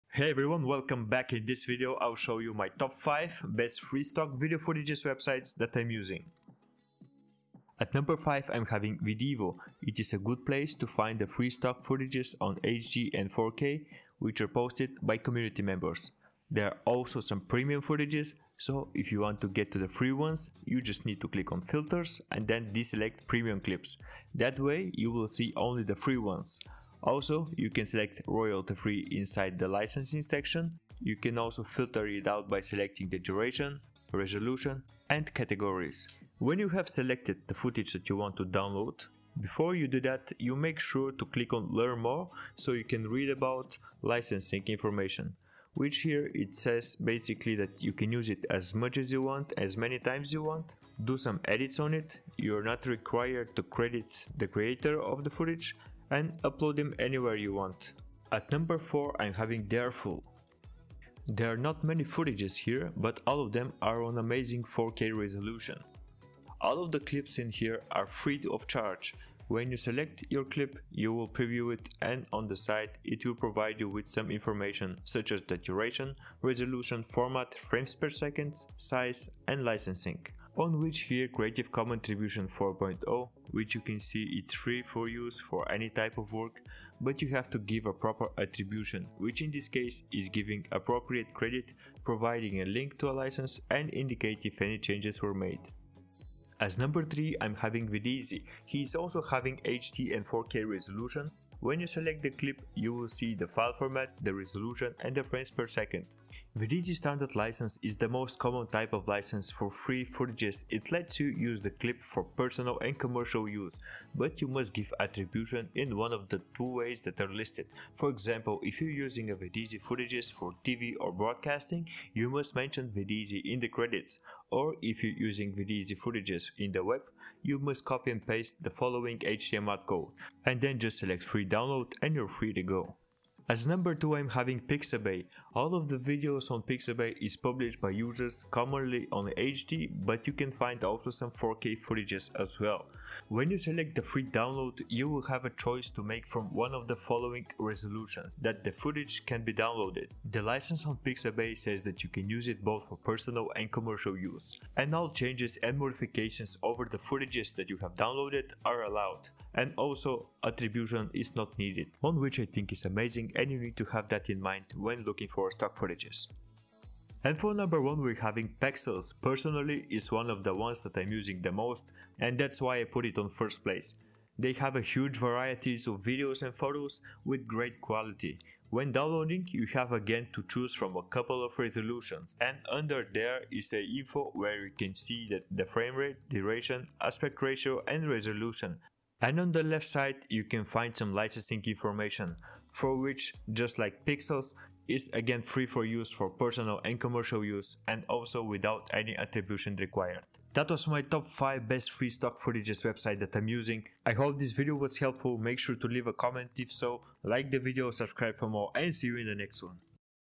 TOP 5 Best Free Stock Footage Websites
Here are my Best Free Stock Video Websites that is use.  They are all high-quality video footage for both personal and commercial use free of charge. In this video, I’ll show  the Top 5 websites i'v used where you can download free stock videos and include those for YouTube,Tiktok,Instagram, commercial use or just for fun.

Top Free stock footage websites photo and stock footage websites